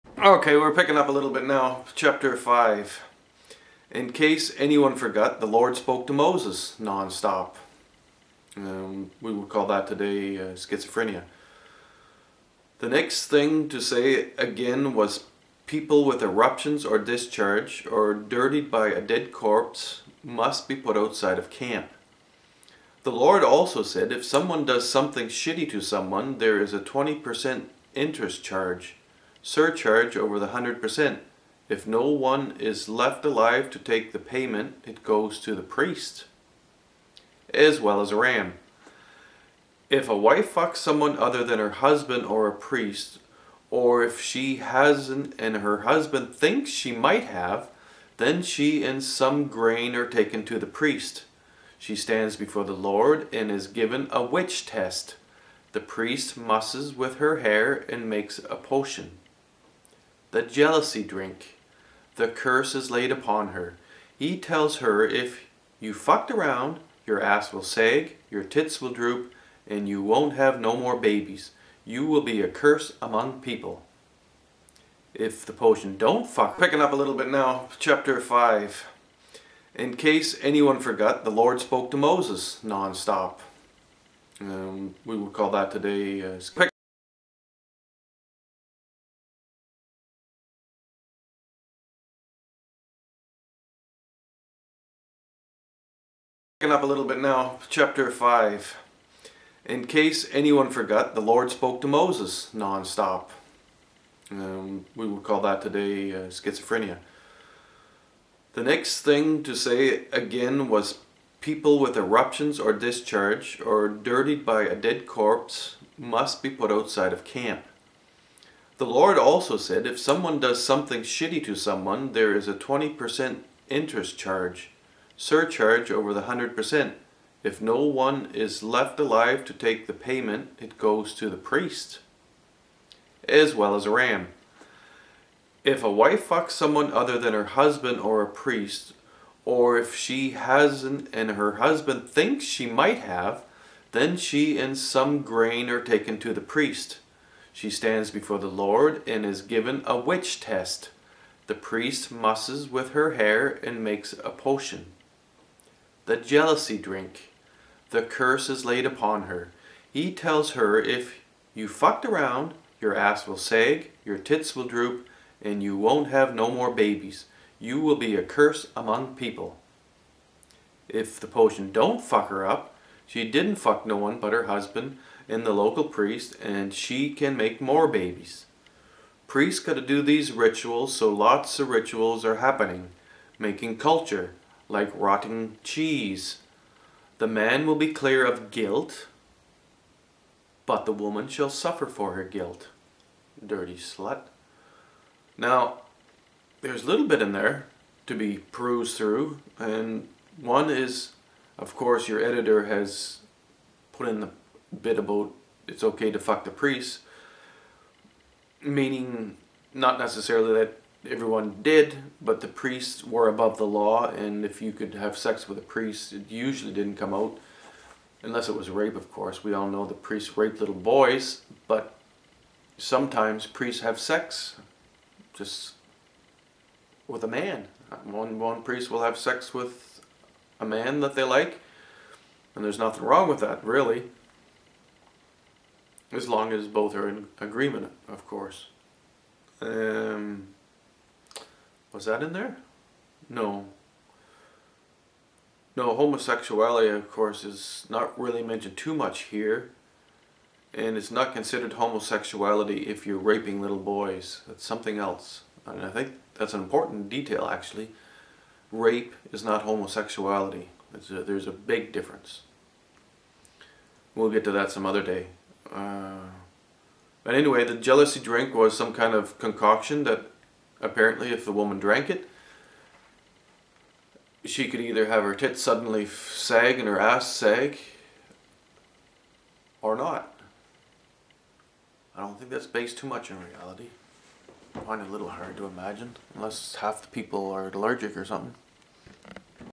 Numbers 05 Steve Howard's New Bloody Torah reading + remarks by John Rah
chapter 05
In case any one forgot, the Lord spoke to Moses. Non stop.
The next thing to say again was people with eruptions or discharges, or dirtied by a dead corpse, must be put outside of camp.
The Lord also said, if someone does something shitty to someone, there is a 20% interest charge. Surcharge over the 100%. If no one is left alive to take the payment, it goes to the priests. As well as a ram.
If a wife fucks someone other than her husband or a priest. Or if she hasn't and her husband thinks she might have. Then she and some grain are taken to the priest.
She stands before the Lord and is given a witch test. The priest musses with her hair and makes a potion. The jealousy drink. The curse is laid upon her.
He tells her if she fucked around, her ass will sag, her tits will droop and she won't have no more babies. She will be a curse among people.
If the poison don't fuck her up, she didn't fuck no one but her husband and the local priest and she can make more babies.
Priests gotta do these rituals so lotsa rituals are happening. Making culture. Like rotting cheese. The man will be clear of guilt. But the woman shall suffer for her guilt. Dirty slut.

peace without belief